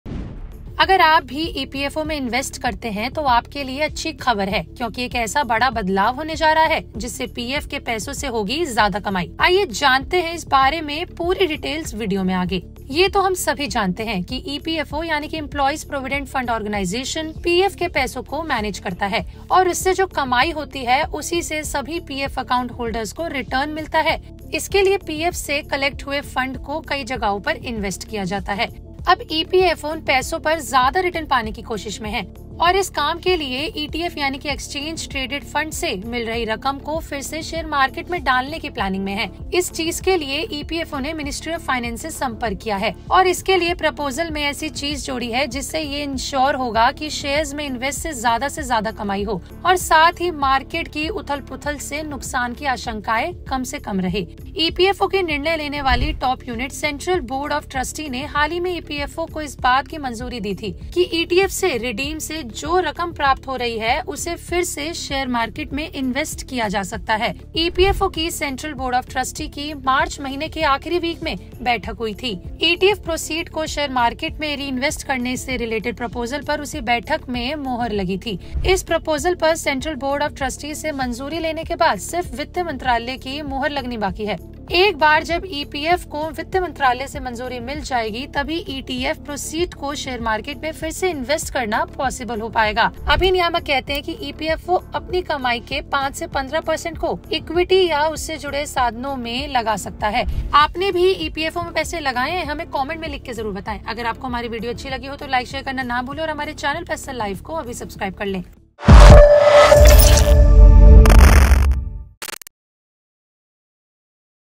EPFO में होगा बड़ा बदलाव, मिलेगा ज्यादा कमाने का मौका । PaisaLive
अगर आप भी EPFO में Accountholder हैं तो ये खबर आपके लिए है वो ये की बहुत जल्द आप अपने EPFO से अच्छा खासा ज्यादा कमा पाएंगे। वो कैसे ये जानने के लिए हमारी इस video ko अंत तक देखें ।